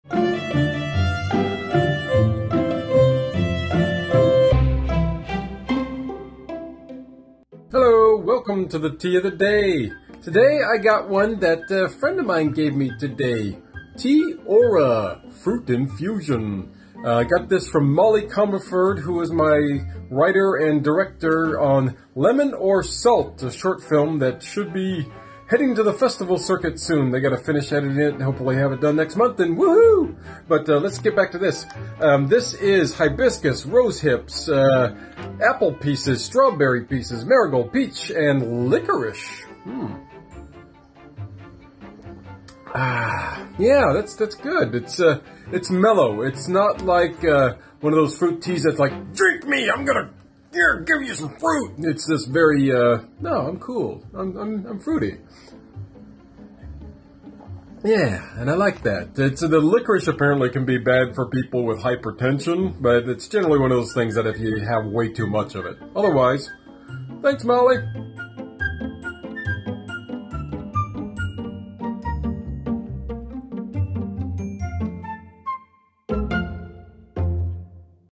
Tea of the Day #38 - Ti Ora Summer Fruits
It's a fruit infusion with Hibiscus, Rosehip, Natural Flavors, Liquorice, New Zealand Apple Pieces (3%), Strawberry Pieces (3%), Marigold, Peach Pieces (1.9%)

Biodegradable pyramid tea bags.

Ti Ora, which means tea of wellbeing, is a tea brand from New Zealand. The thing is, in New Zealand we have a really close relationship with nature. It's part of who we are. And we believe that most people could use a little more of that stress-free, easy-going state of mind that the great outdoors brings.
Our teas are made from high-quality natural ingredients, complemented with New Zealand Manuka botanicals. So if you want a little bit of that NZ mindset, just pour yourself a cup.